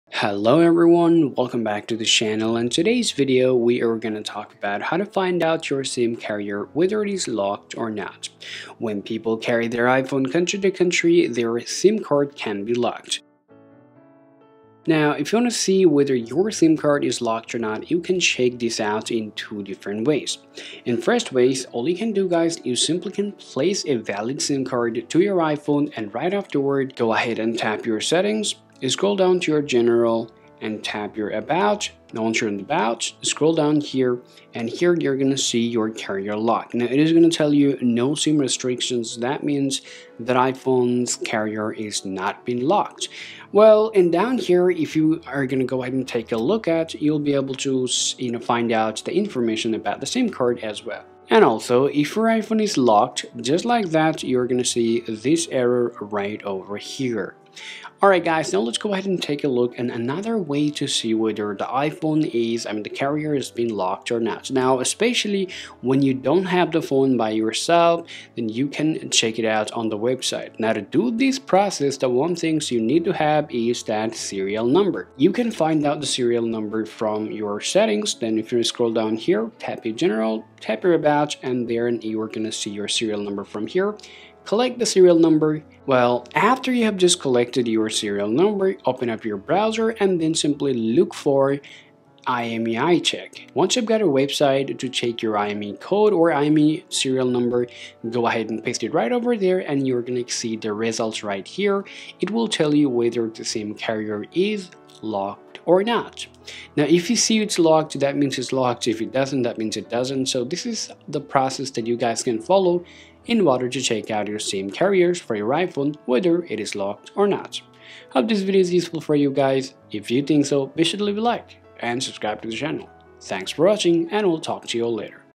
iPhone Carrier Lock or Unlock? - Check Here with or without SIM!
If you are purchasing a used iPhone, you have to know if the iPhone is carrier unlocked or locked. Many people ask how they can check that. In this video, we will show how you can check if the iPhone is carrier-locked or unlocked with or without Sim card completely free.

0:00 Opening
0:15 Check iPhone Sim Card is locked or not
0:58 Check iPhone is locked or not from Websites